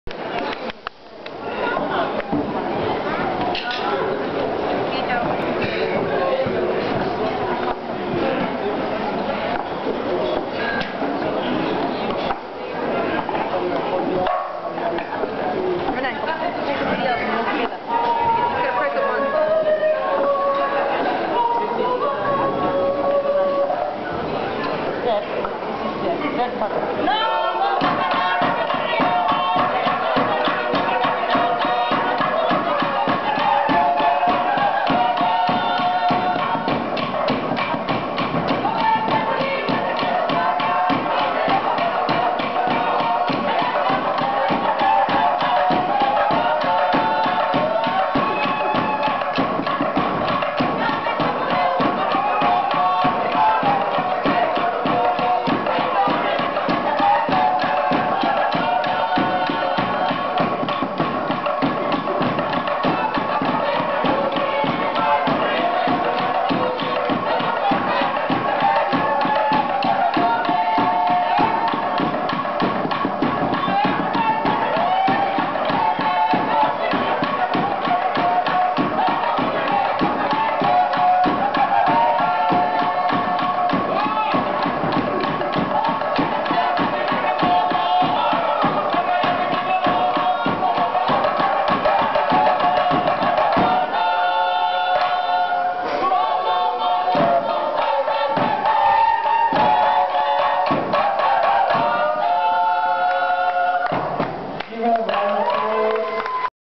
Fijian Meke performance
Performed by Korotogo performing group at Outrigger on the Lagoon resort, Sigatoka, Fiji.
November 2012